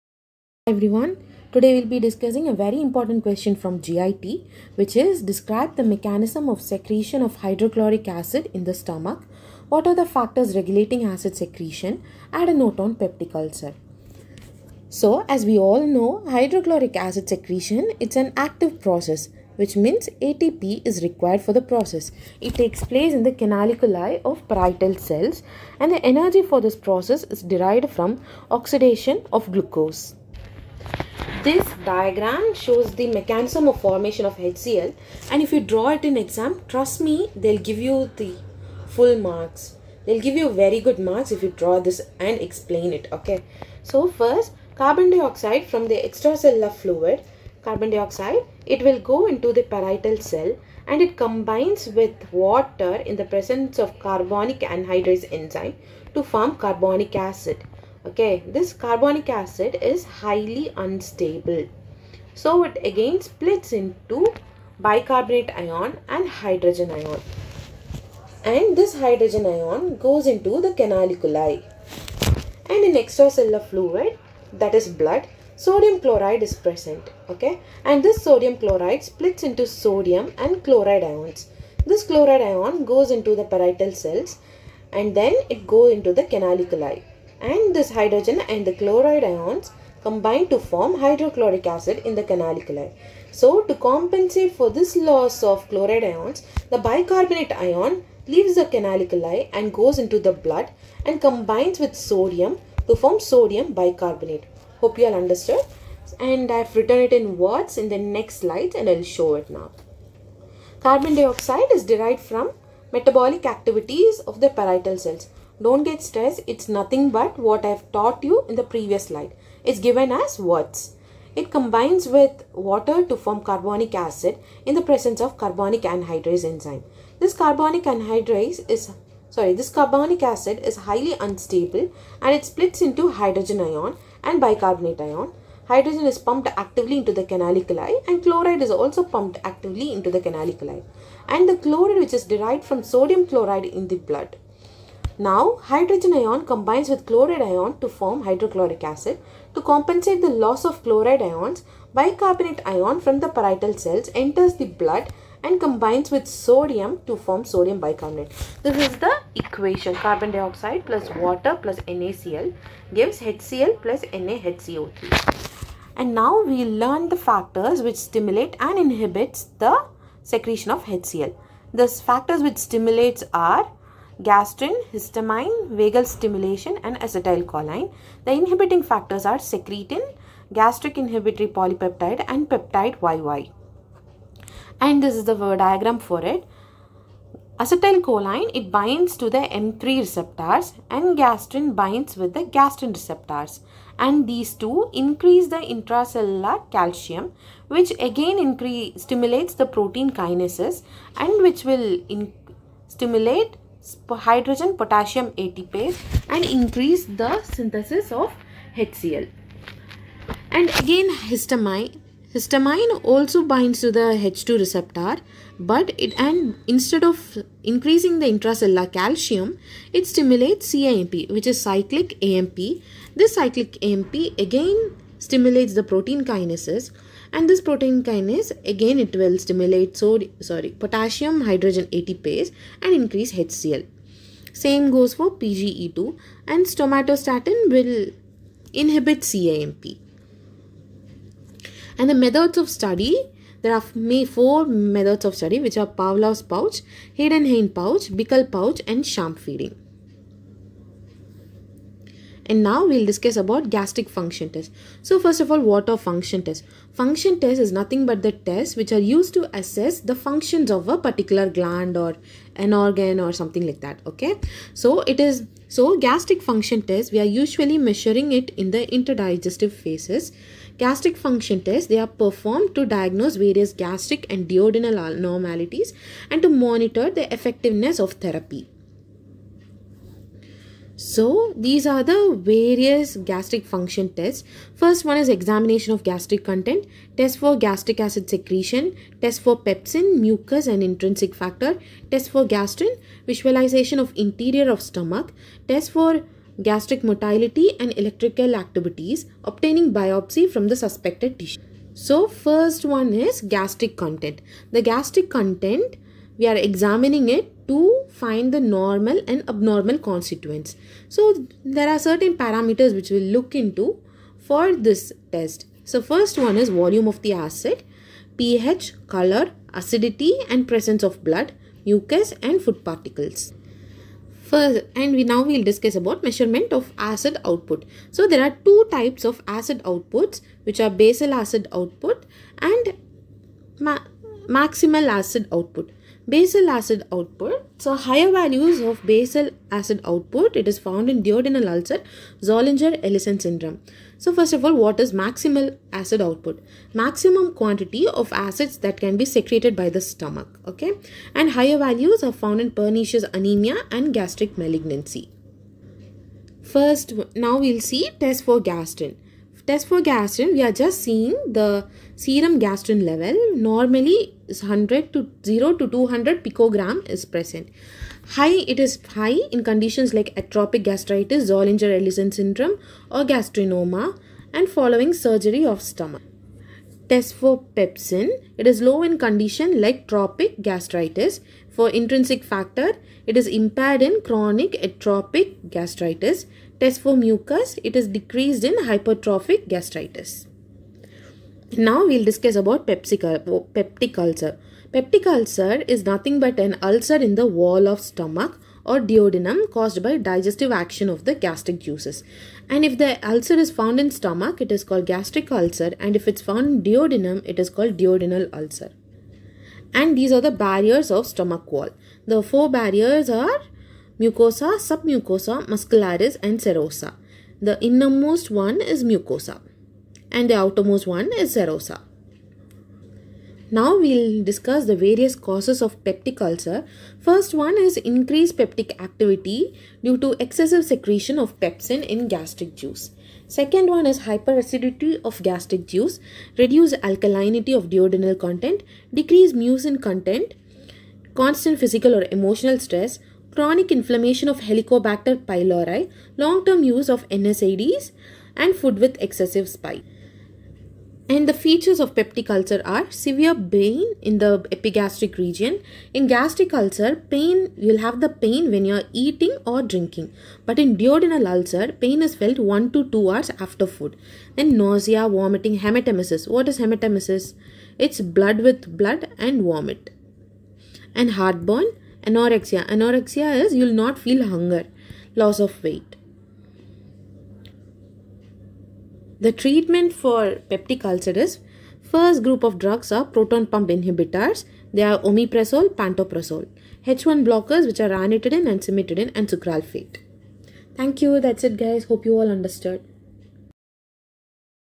HCL Secretion - Stomach | GIT | Physiology | 1st MBBS | Revision Video | PYT | Dr. Agam
A video series for 1st year exam going batch, to escalate their exam preparation. Grab your opportunity and enhance your preparation today!

Topic : HCL Secretion
Presenter : Hannah Aishwarya